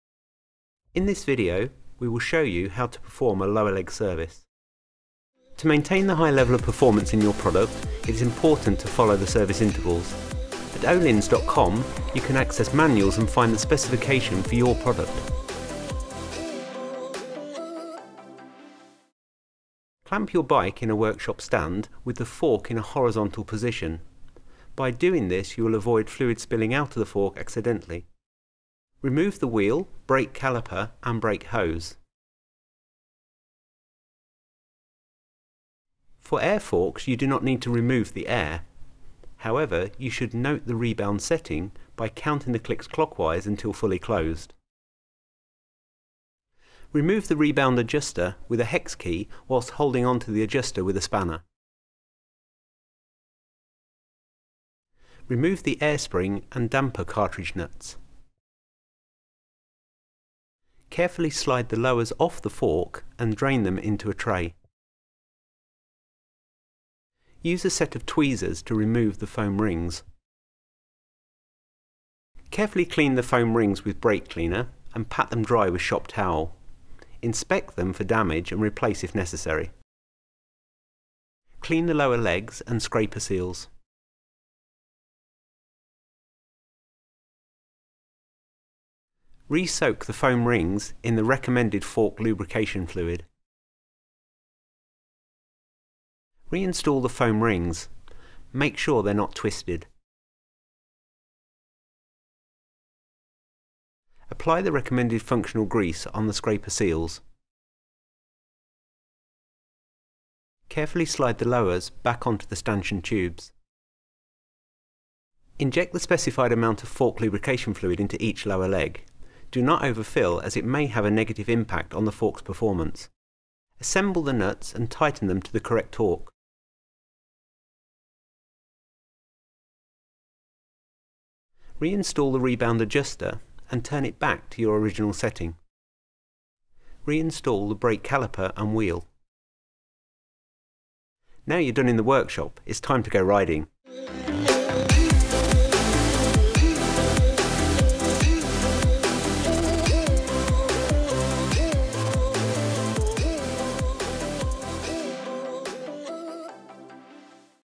50 hour lower leg service on an Öhlins RXF36 m.2 Air fork
How to perform a 50 hour lower leg service on a RXF36 m.2 air fork. The operation is similar for the original RXF36 and RXF34, make sure to follow the workshop manual.

Fluids needed
01336-01 - Renep CGLP 68 Fork lube 1L
01338-22 - Renolit SI 410 M Silicone grease 225g

Here you can find the workshop manual with a detailed description of parts, tools, fluids and torques